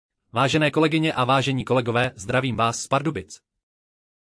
Porovnání různého nastavení D-ID při generování videa
Online aplikace D-ID umožňuje při generování videa z fotografie nastavit různé parametry. Video vzniklo jako pomůcka k příspěvku DigiDay 20 17. dubna 2024.